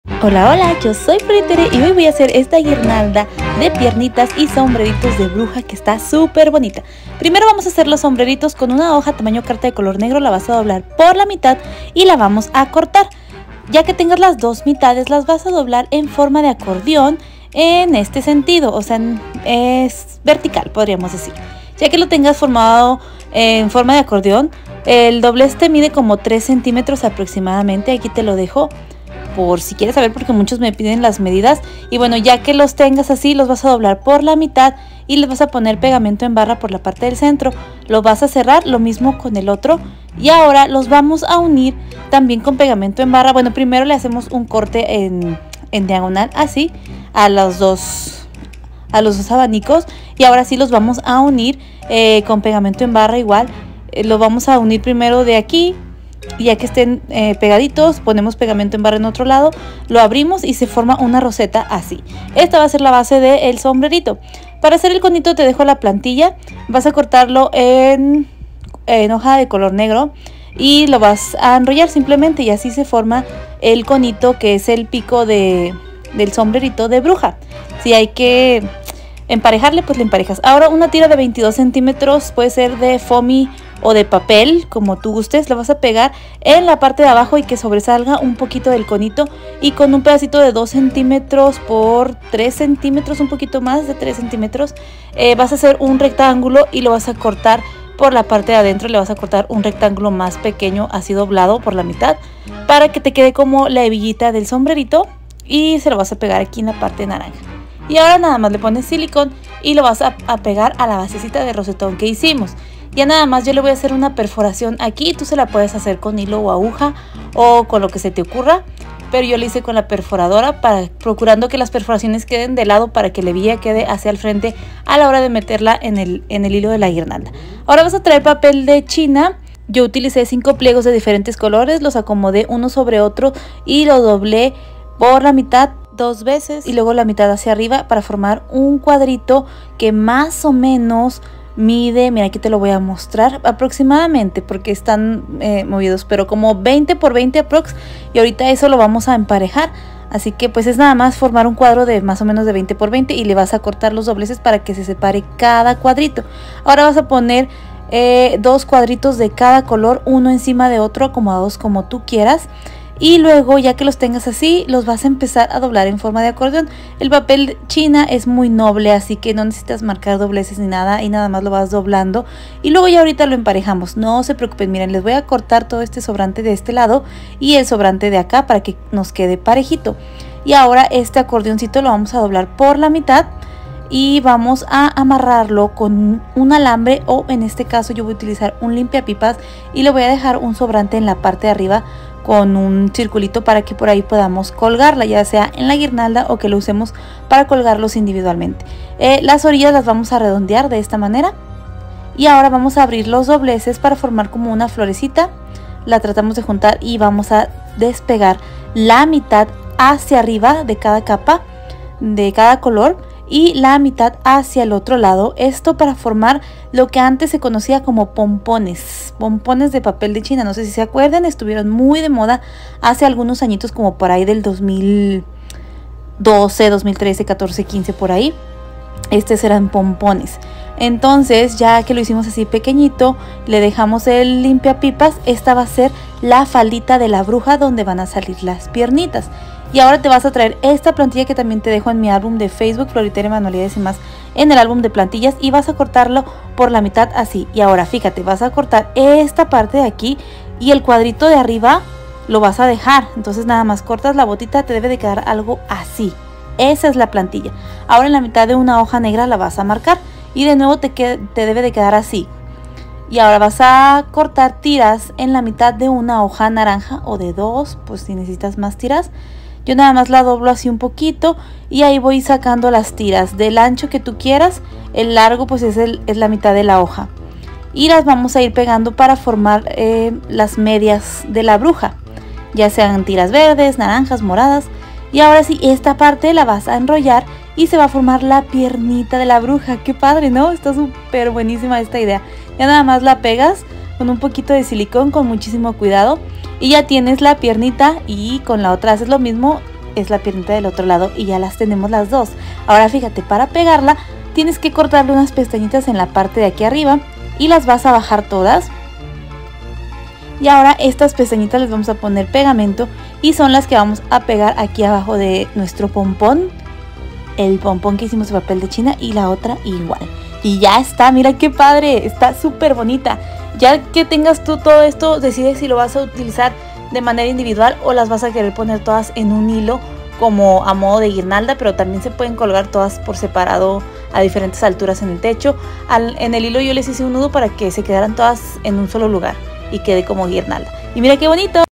Guirnalda de Piernas y Sombreritos de Bruja 🧙🏼‍♀️
Manualidades con papel para halloween, decoración para halloween piernas y sombreros de bruja
How to make halloween paper garland Witch hat, Witch legs